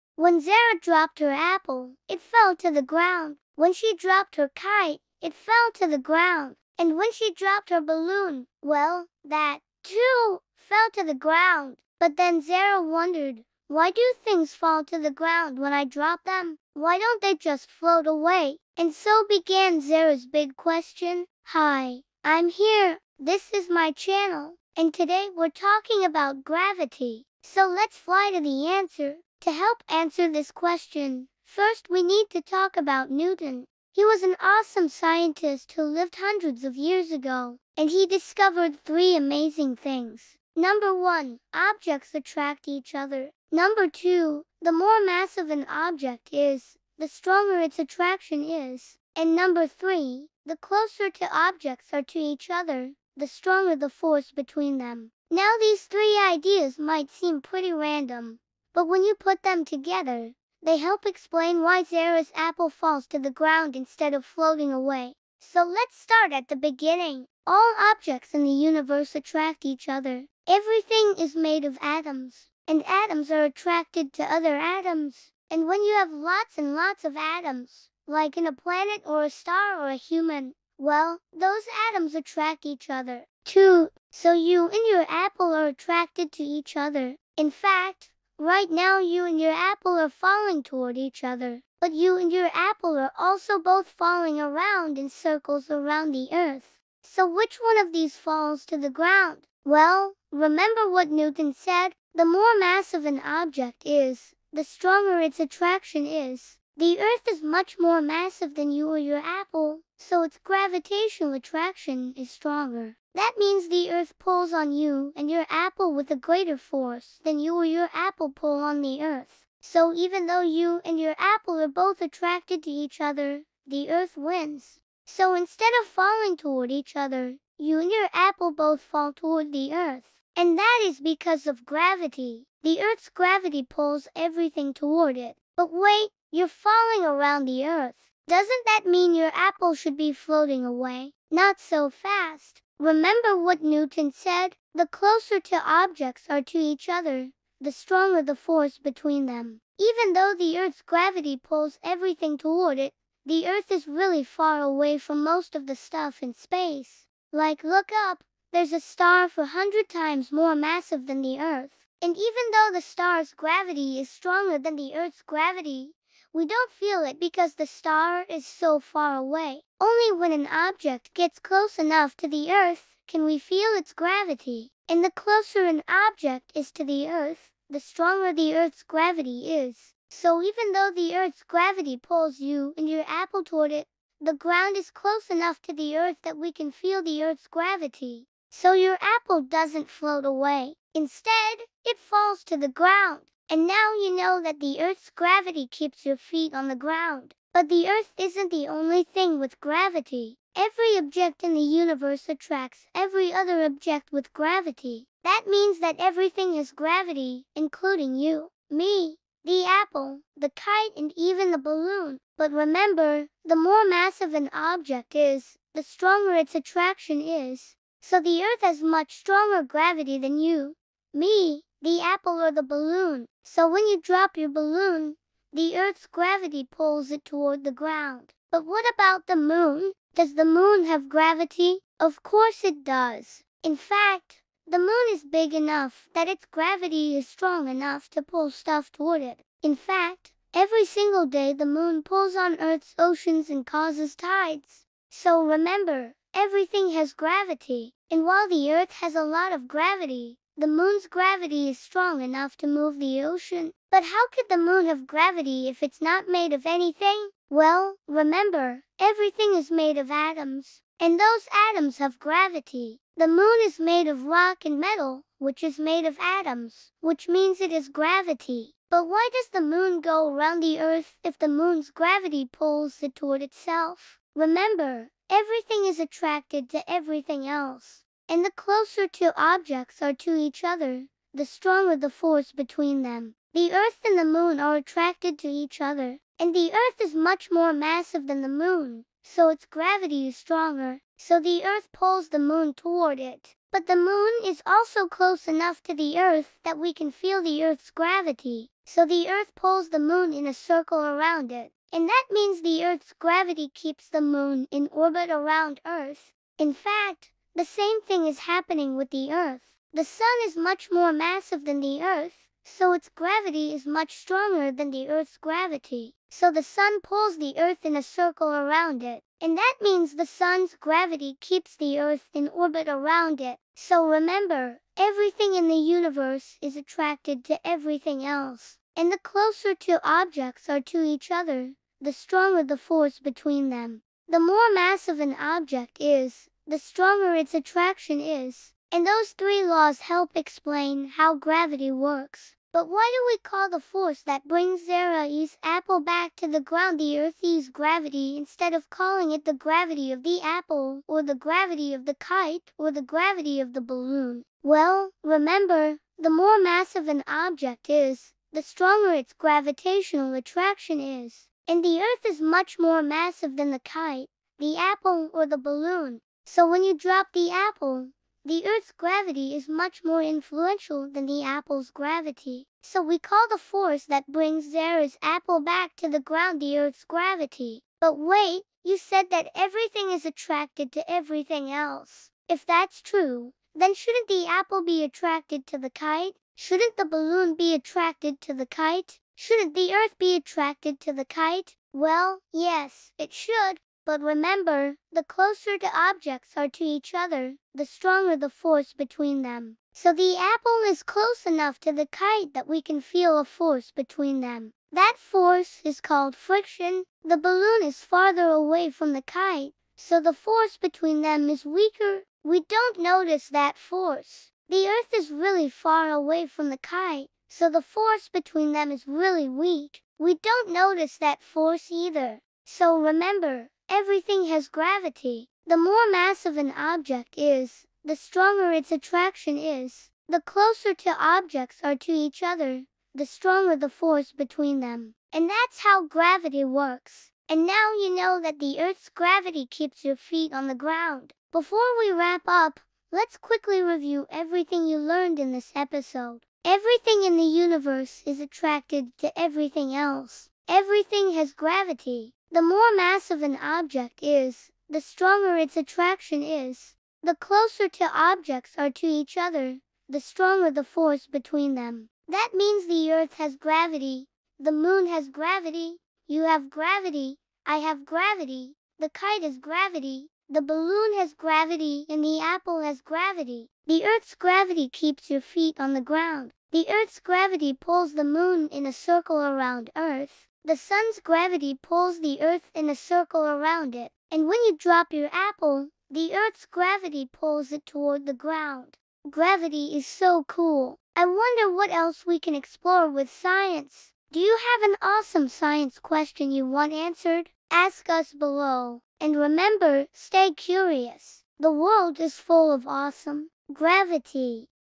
What Is Gravity? 🪂 | Mini Science with Zara and Leo | Fun Physics for Kids
Why do things fall down and not up? 🤔
Join Zara and Leo on a fun-filled science adventure as they meet Gravi — a friendly floating guide who teaches them all about gravity!

In this exciting episode of Mini Science for Kids, children will explore one of the most important forces in the universe in a simple, story-based way that makes learning fun and unforgettable. 🌍🧠

🎓 Kids Will Learn:
✅ What gravity is
✅ Why things fall down
✅ How gravity keeps us on Earth
✅ How gravity works in space

Perfect for preschoolers, early learners, and curious minds everywhere!